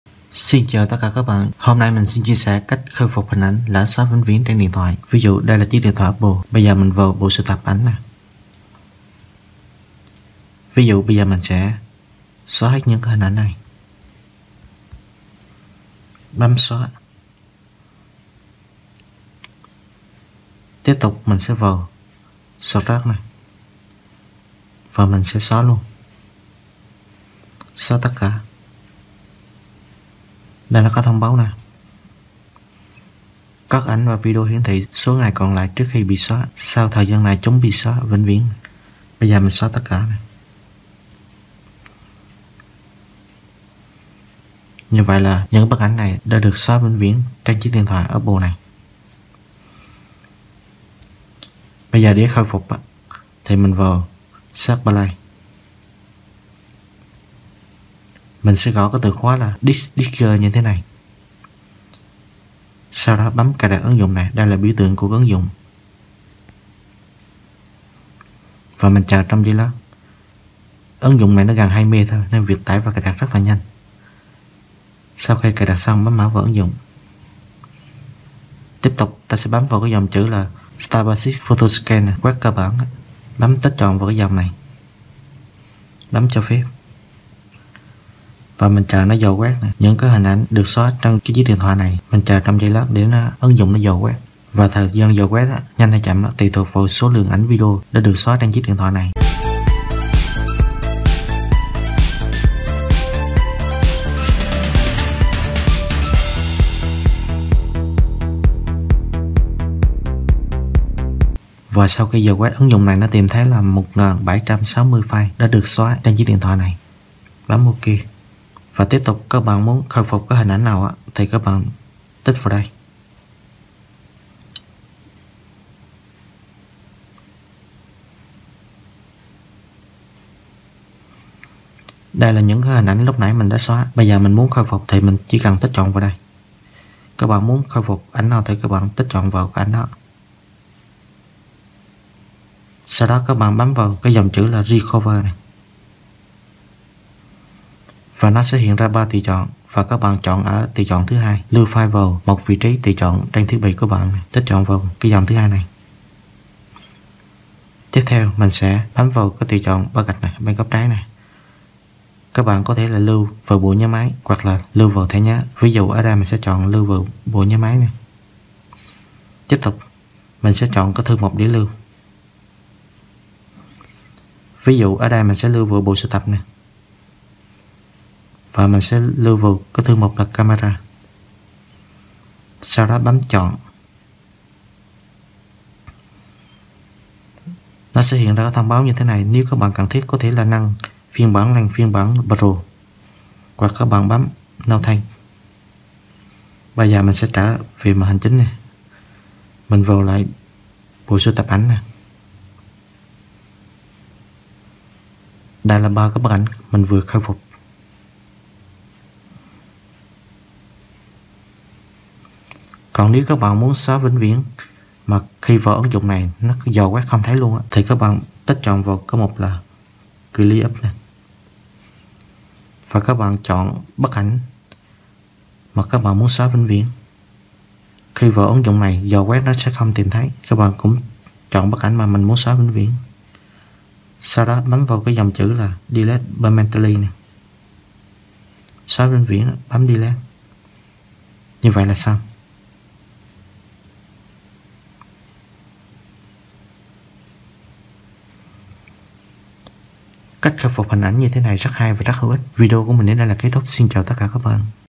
Cách khôi phục hình ảnh , video đã xóa vĩnh viễn trên điện thoại || Cách lấy lại ảnh đã xóa
Video chia sẻ cách khôi phục ảnh đã xóa vĩnh viễn trên điện thoại
Cách lấy lại hình ảnh đã xóa
Cách lấy lại video đã xóa